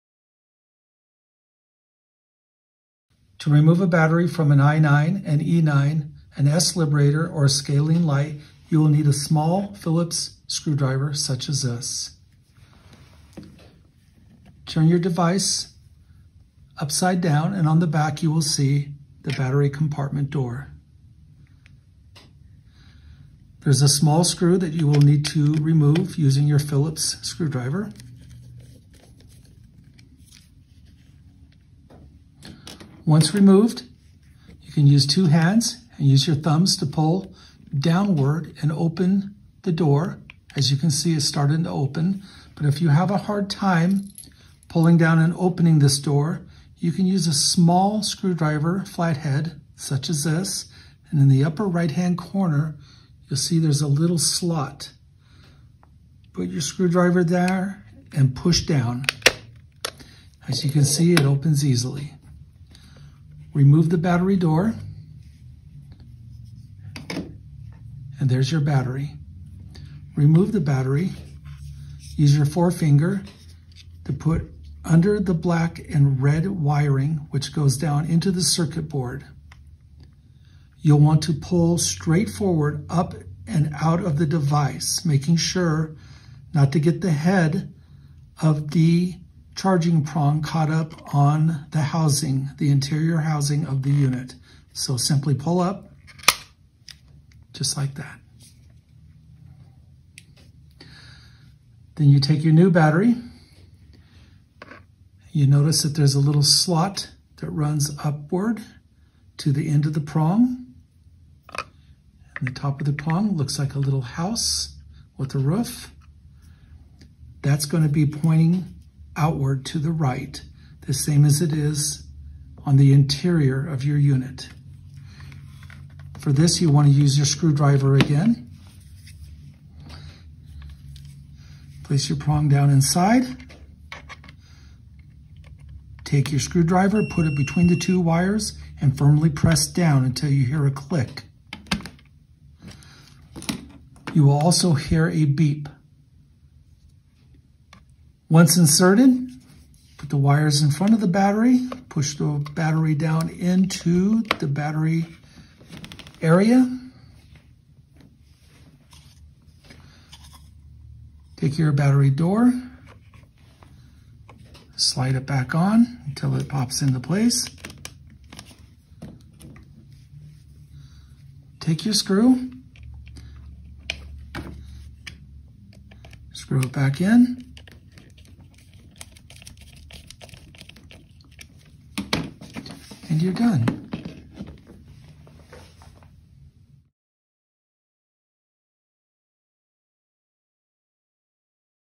How to Replace the Battery in a I-9, E-9, S-Liberator or Scalene Light
This video guides you through the process of replacing the battery on the CHI Infratonic I-9, Equitonic E-9, S-Liberator or Scalene Light.